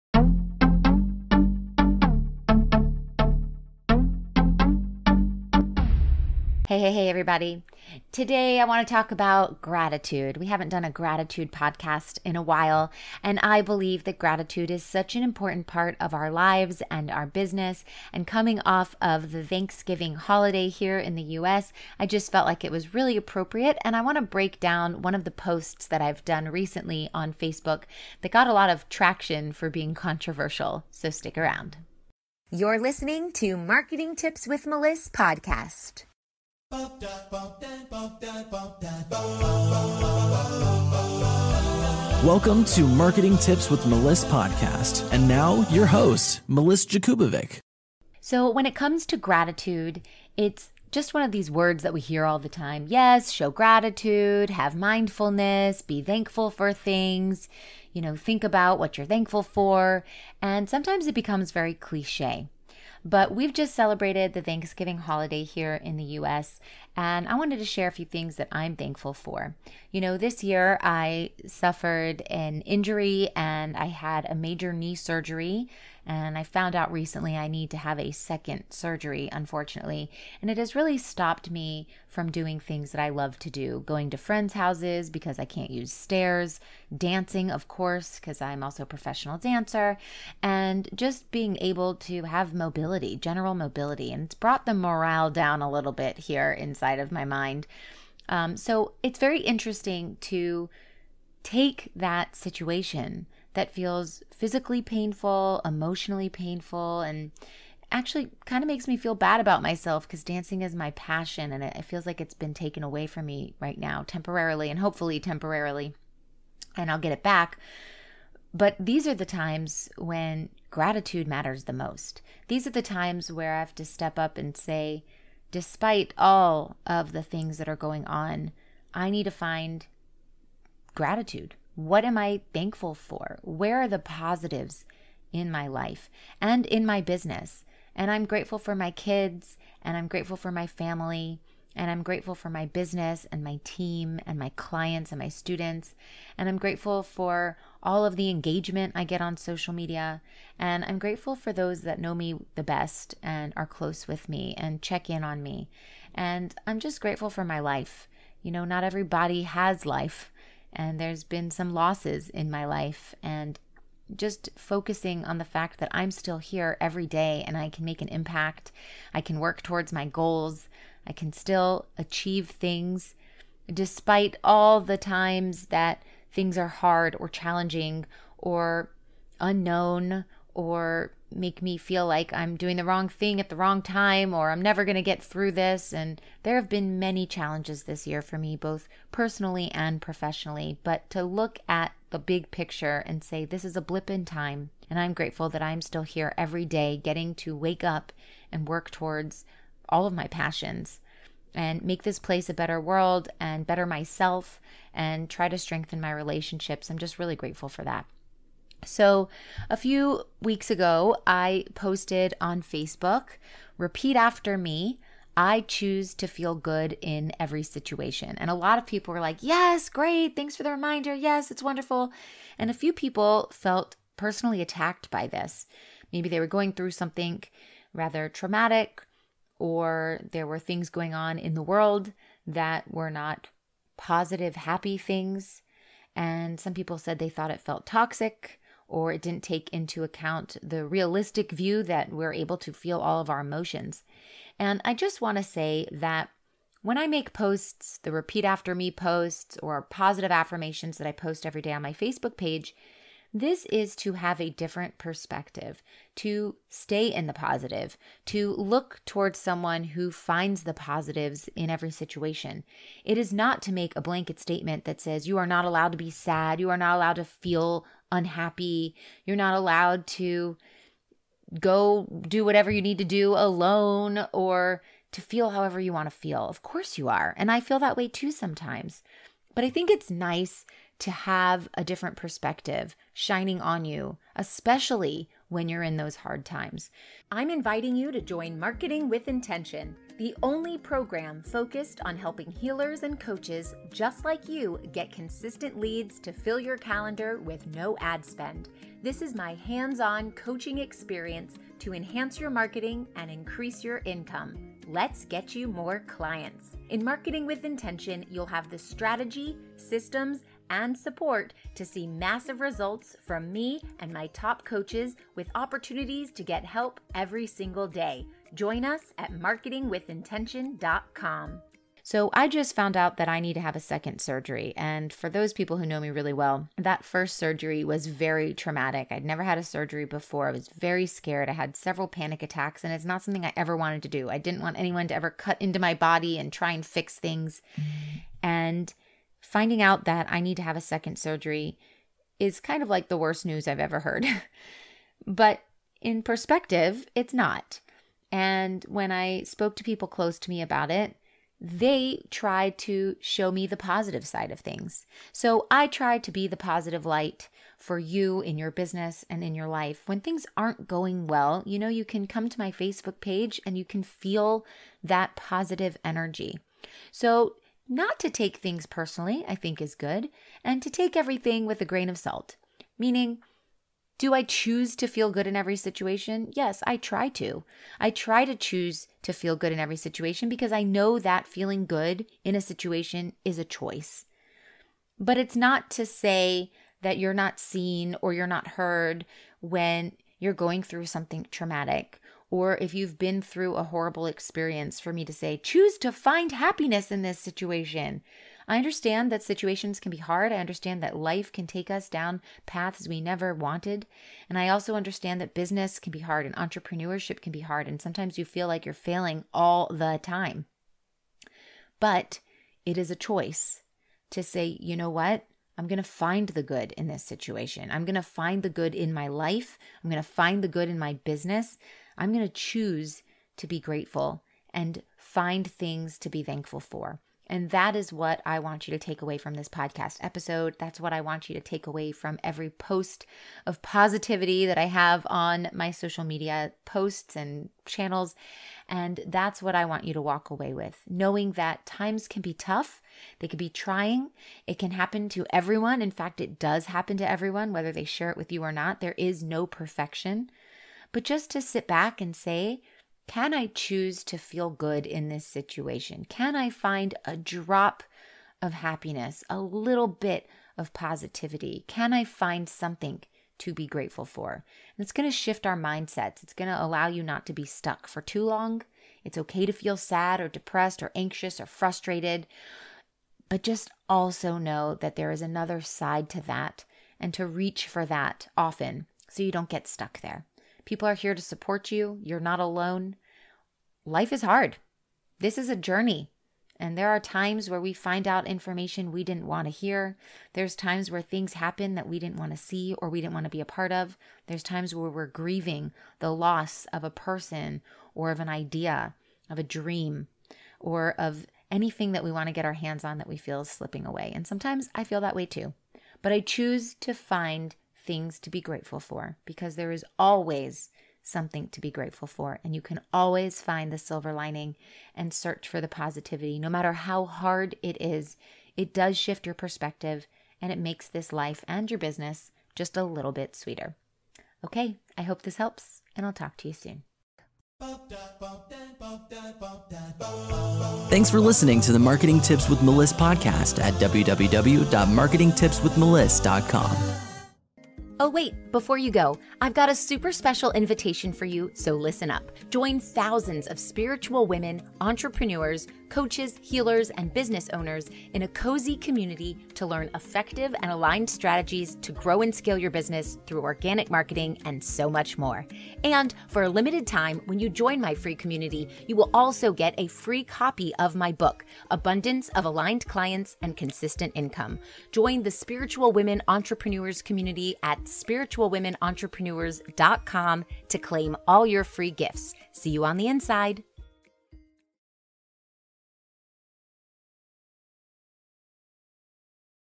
Marketing Tips With Meliss-Ep.446: Gratitude Is Always In Style
"Take everything with a grain of salt."

In this episode of Marketing Tips With Meliss, I'm talking about gratitude:

-Where are the positives?
-Life can take us on paths we never wanted, but it is a choice to say "I'm going to find the good." Choose to be grateful!
-There is no perfection. Can you choose to feel good in this situation?

Connect with Meliss Jakubovic:

Learn more about how Meliss helps Coaches and Healers add $10K+ to their monthly revenue at melissmarketing.com